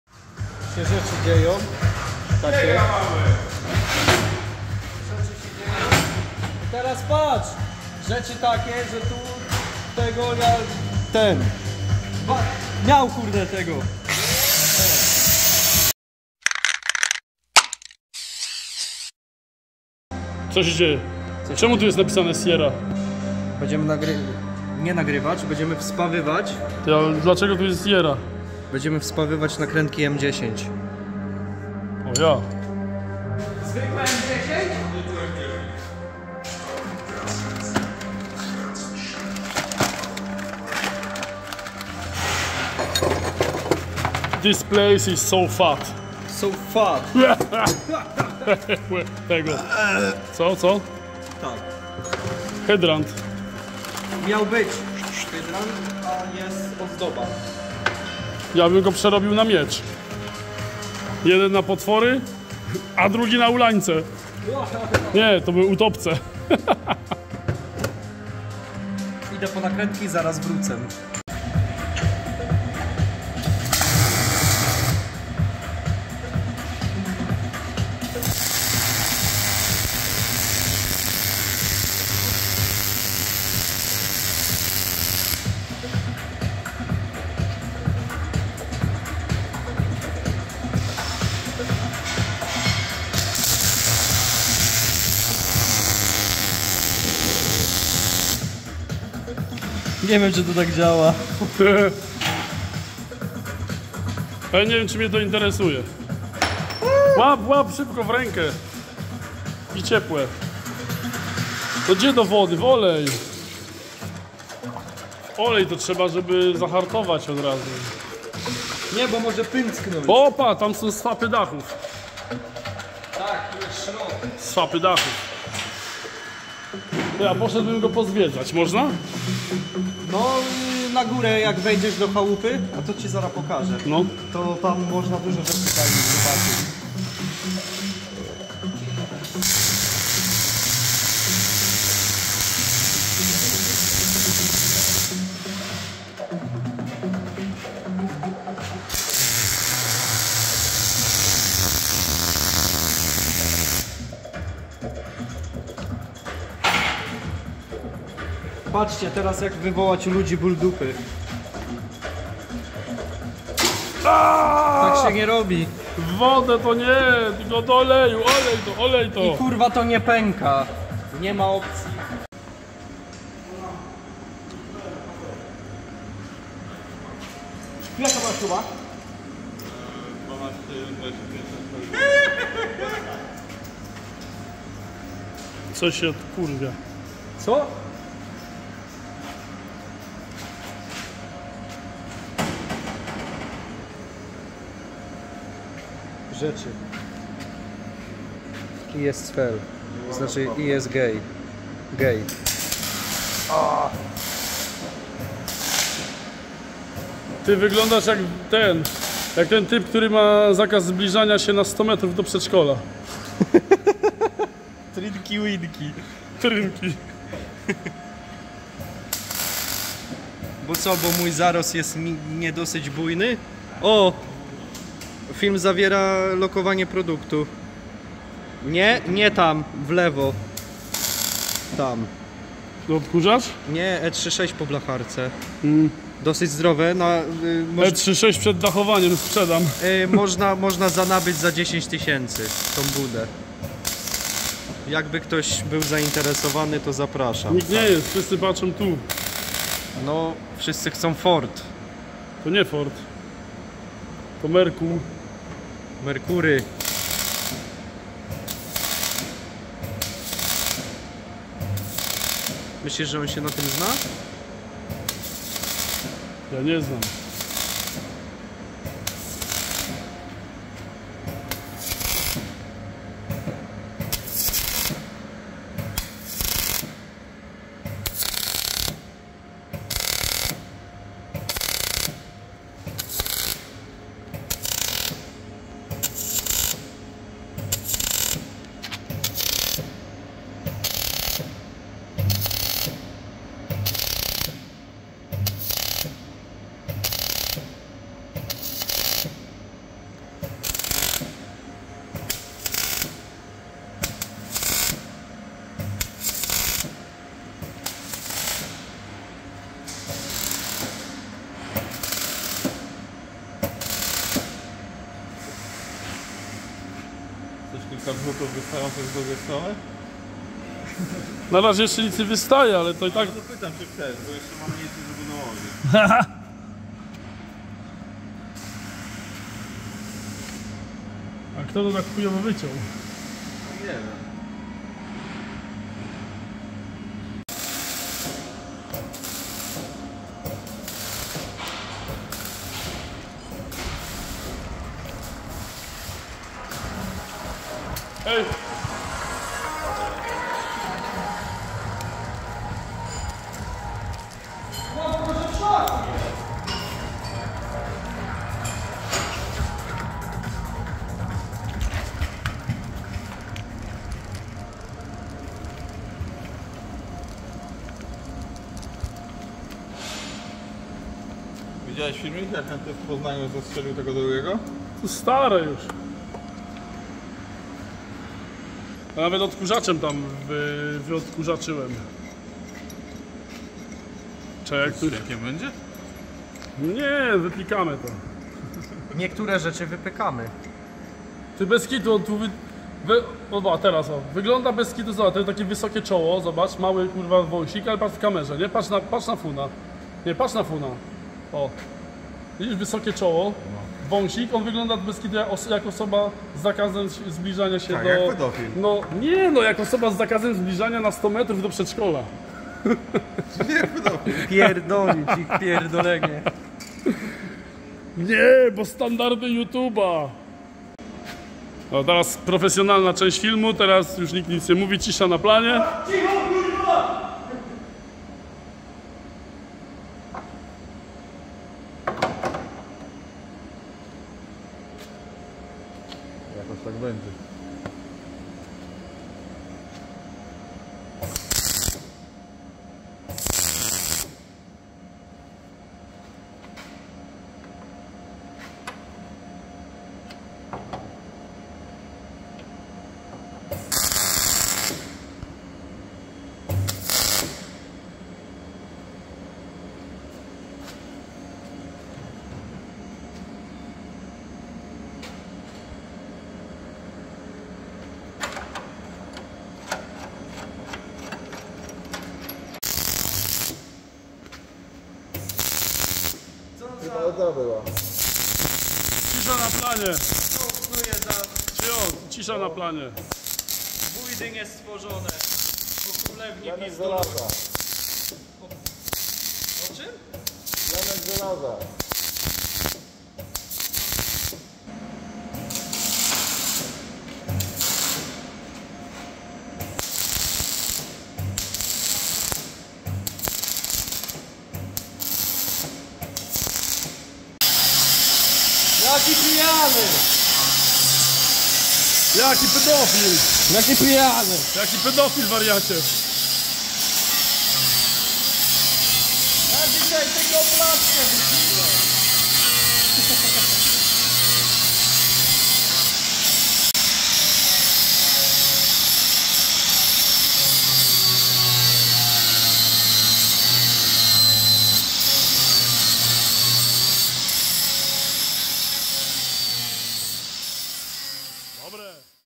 Bandyci z Funo Auto Tuning psują mi Merkur Sierra
W tym odcinku Adam jest z wizytą w FAT żeby zobaczyć jak tam jego Merkur Sierra